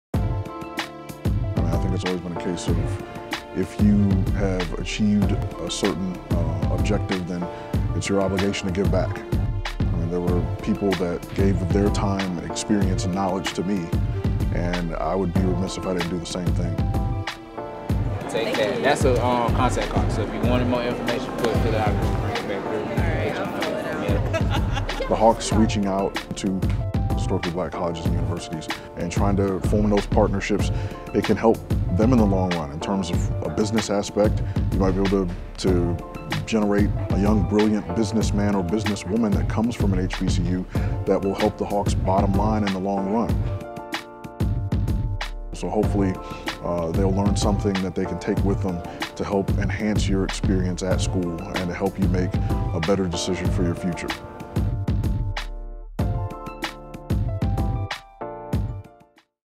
Georgia Power teams up with the Atlanta Hawks for HBCU Day
On Martin Luther King Jr. Day 2018, Georgia Power Teamed up with the Atlanta Hawks to host HBCU Day. Over 200 high school and college students attended the event and were able to complete college applications at no cost and speak with representatives from various companies in Atlanta for future career opportunities.